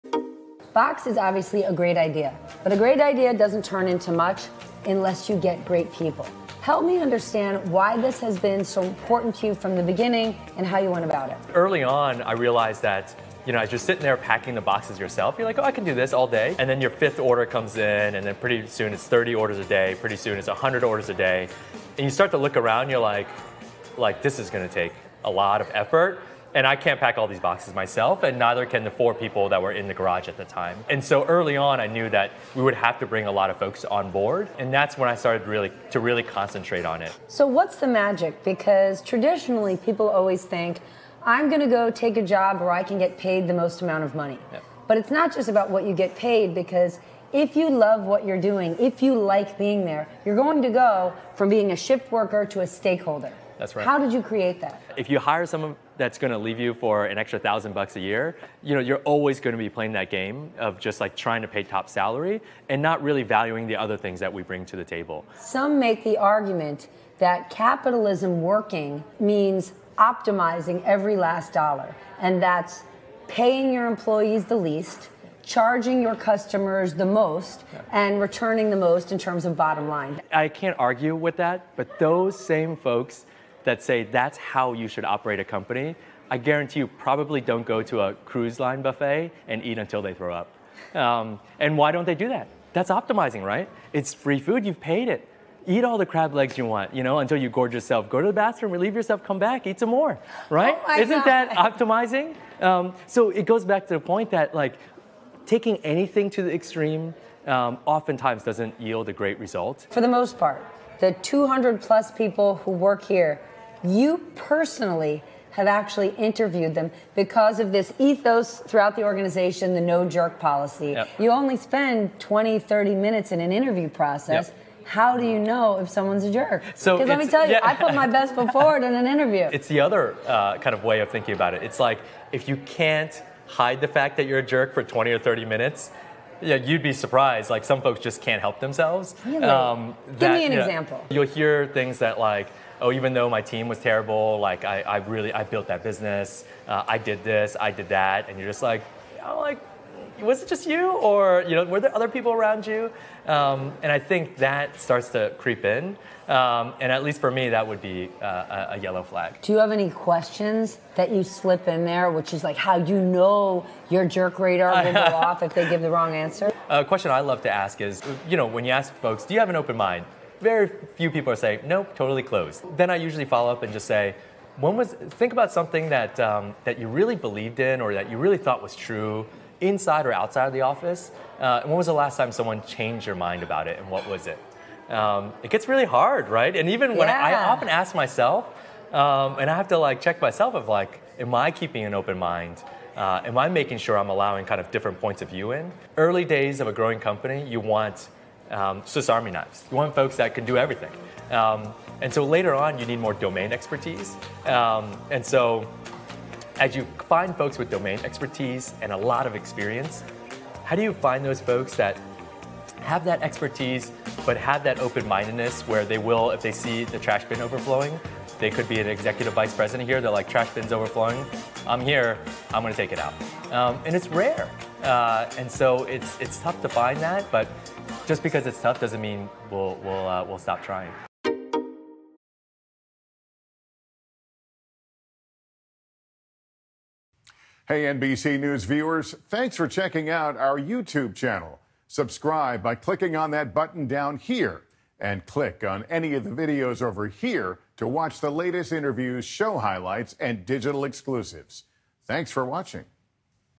Boxed CEO: How To Avoid Hiring Jerks | NBC News Now
Hiring can be tough and making sure your employees are team players is essential to success. In this week's Better Business conversation, Stephanie Ruhle talks with Boxed CEO Chieh Huang about his techniques for making sure he doesn't hire jerks.